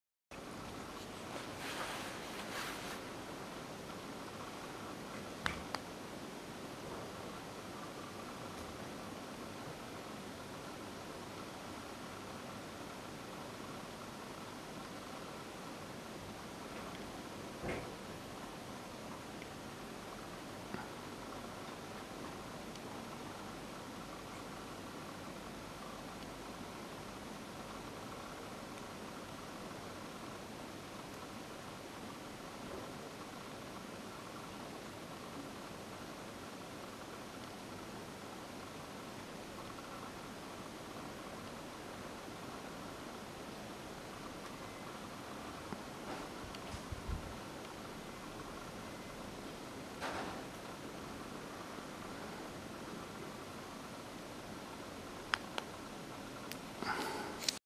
Led LenserX14の威力(スポットライトテスト)
約35m先のマンション投影です